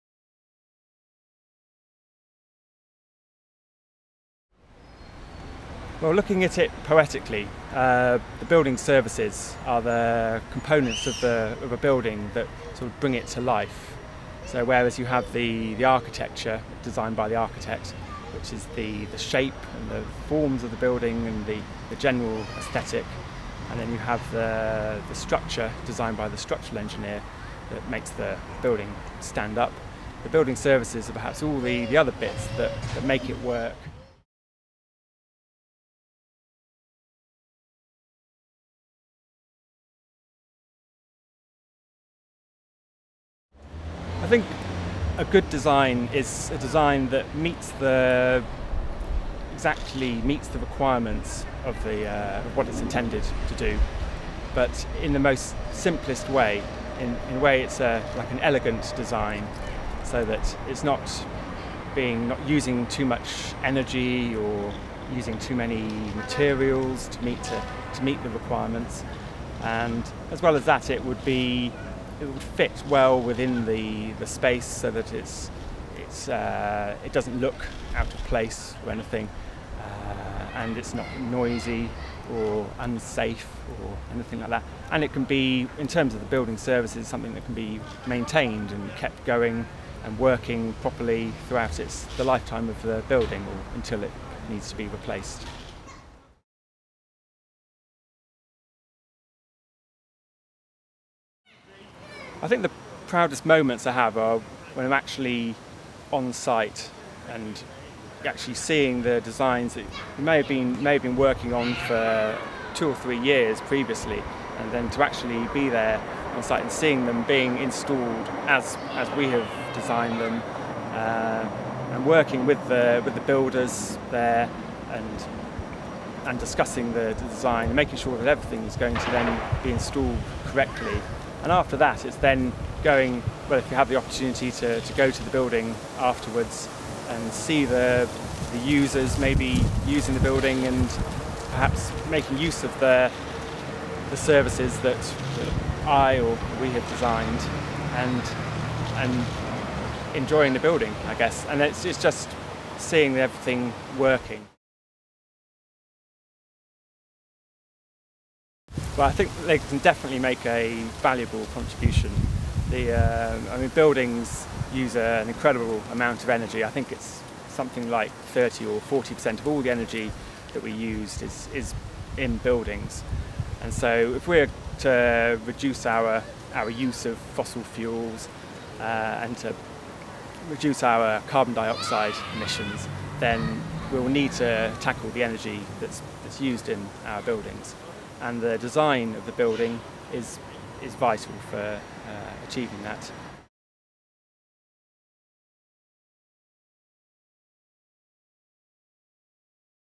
CIC_Careers_BuildingServicesEngineer.mov
This film explores the role of the Building Services Engineer in construction and has been produced by the Construction Industry Council as part of A Professional Career in the Built Environment. To download a free careers pack visit
and click on Information Advice and Guidance. CIC would like to extend a a special thank you David Hawkins of Max Fordham and Fiona Boyce of Mace for taking part in this series.